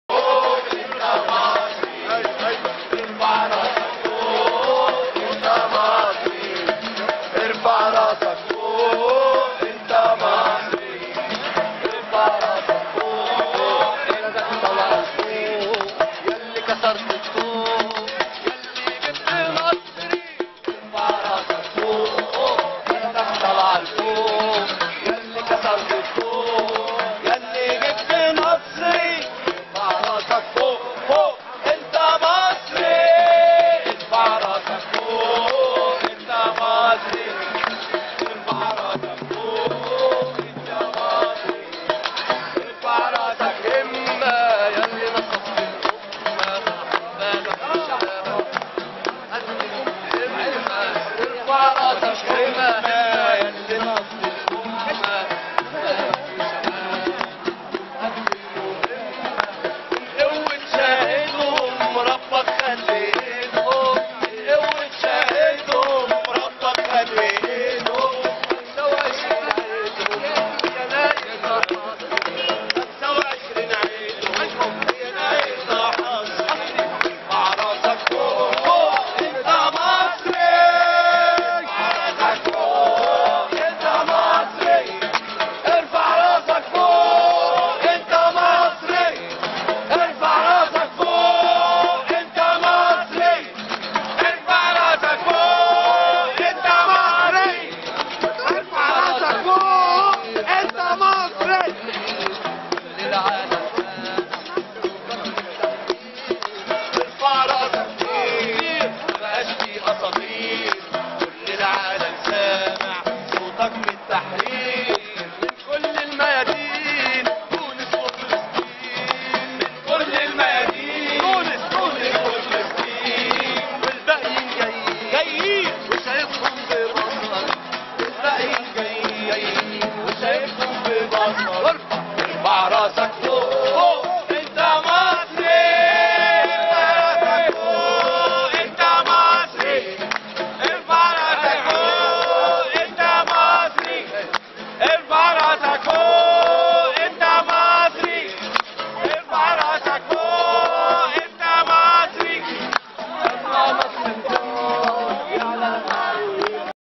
Egypt مسيره جبهة الابداع مصر 2012
Egypt مسيره جبهة الابداع للدفاع عن الحقوق ف الفن و التعبير و الابداع و حرية الكلمه و الفكر من دار الاوبرا الى مجلس الشعب .23 يناير 2012 القاهره مصر و شعارها مصر مصريه